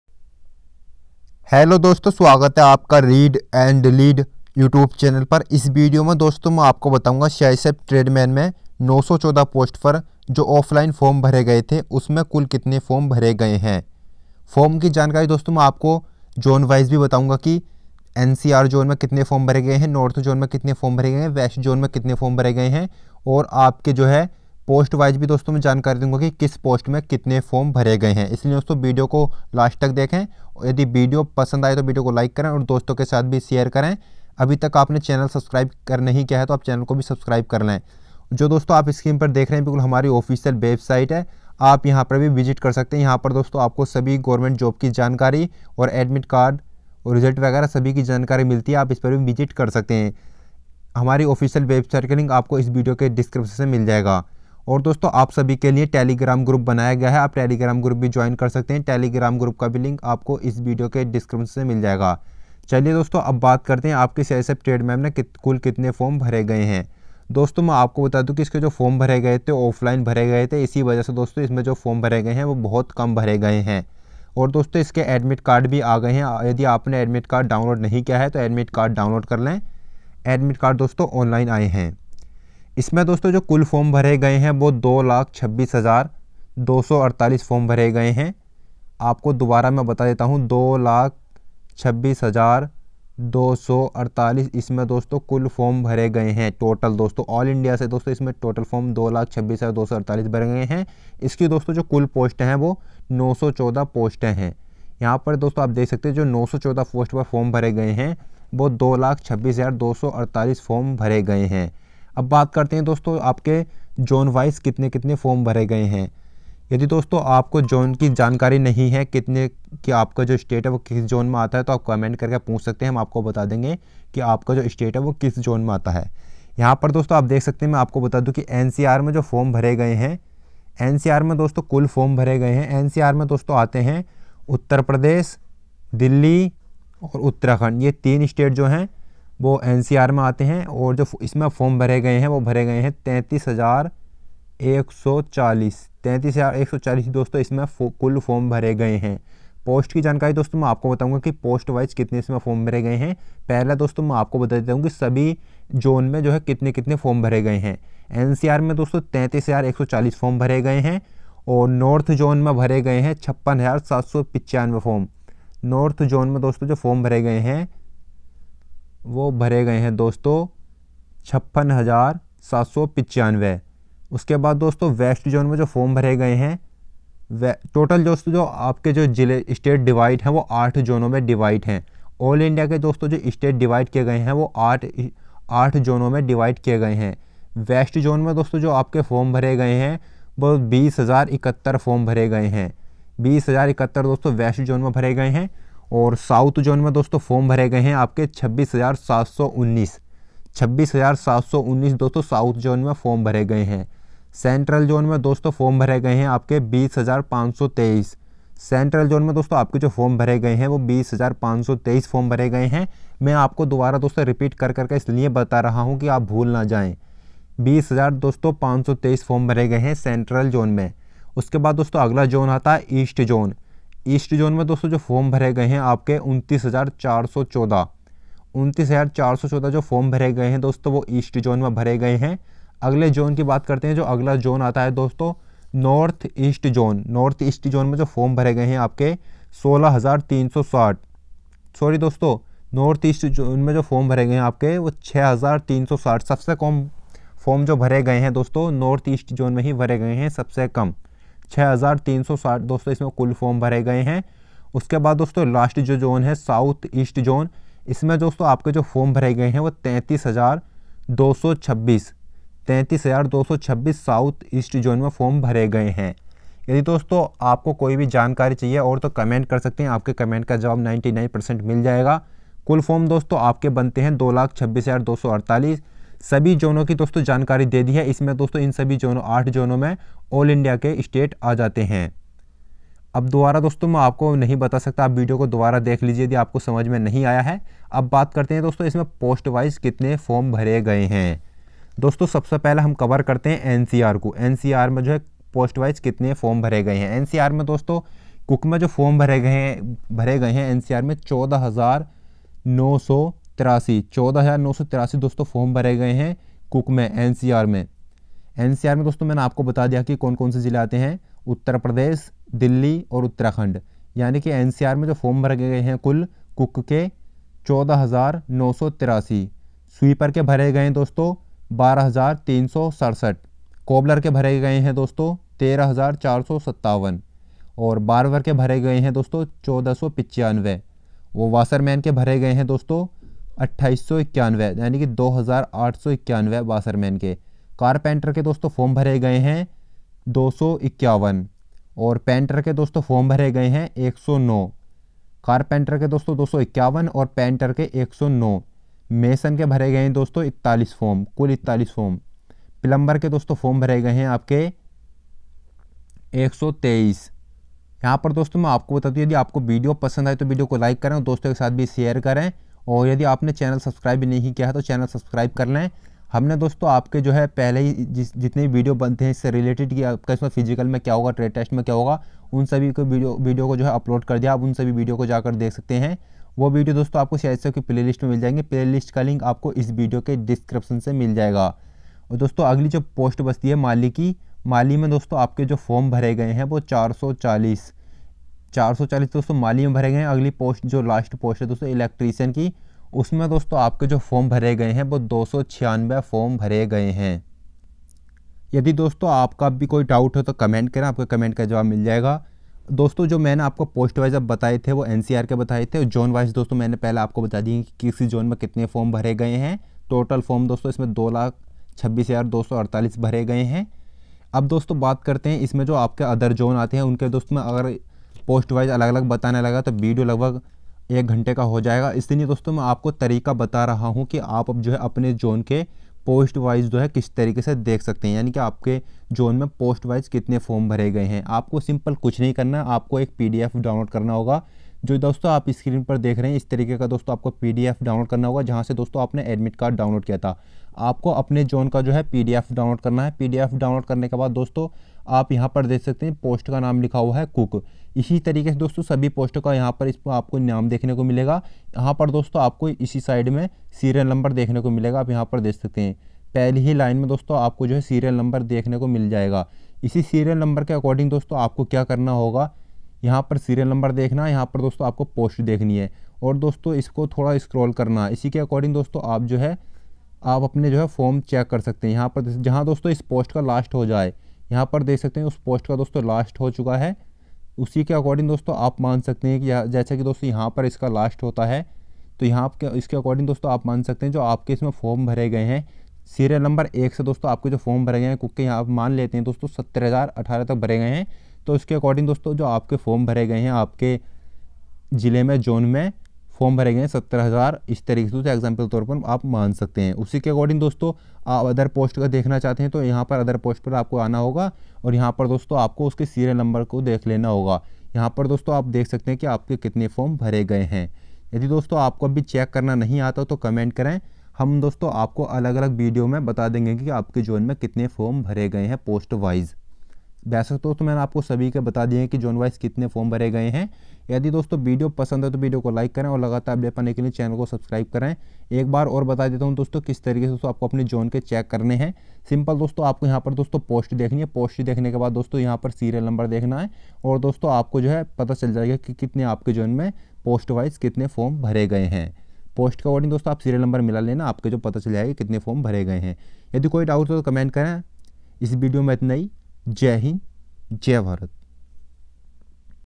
CISF Trademan में कुल कितने आवेदन किये गये हैं। || CISF Trademan में कुल कितने फार्म भरे गये हैं।
CISF Trademan zone wise form 2020

CISF Cook Trade Test 2019 || CISF Trademan में Cook Trade Test कैसे होता है जाने। Complete जानकारी